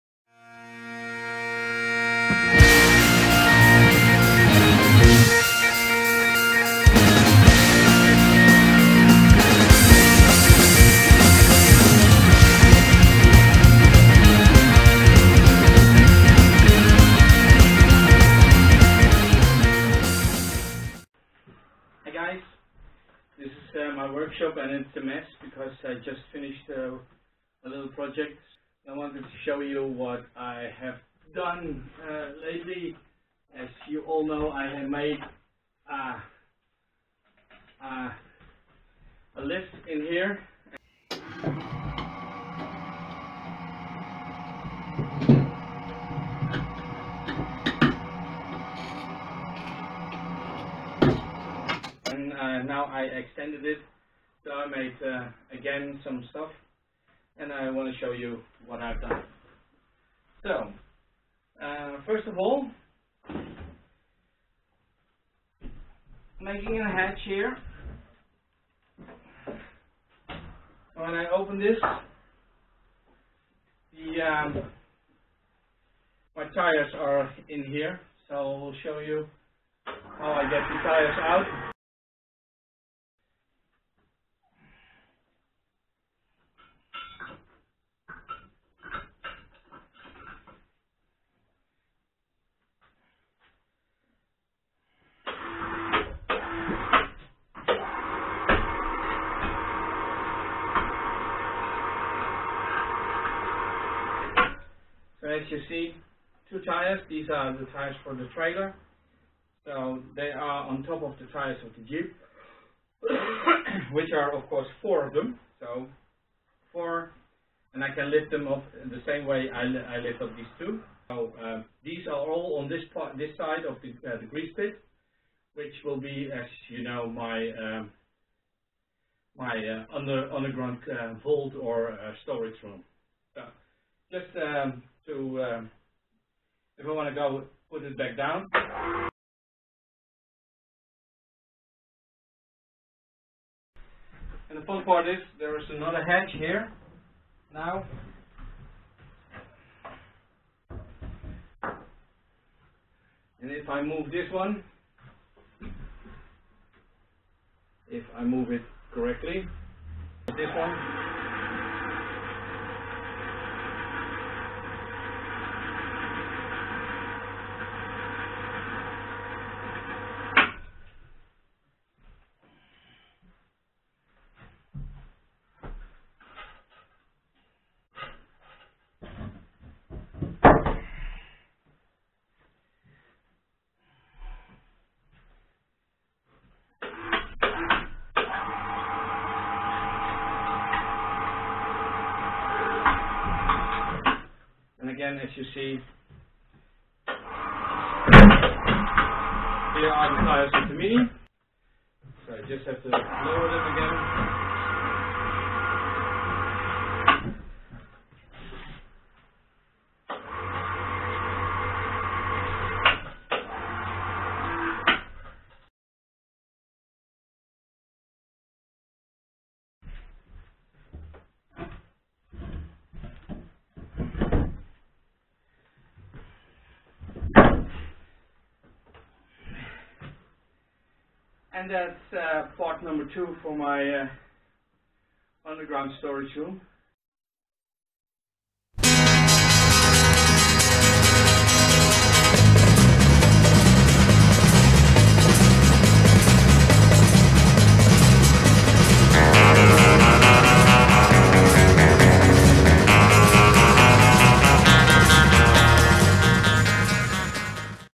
Dragonriders Travel Under ground Storage room part 2
Making my grease pit into a storage room. With a winch I can hoist the closet and the tires up from below and back again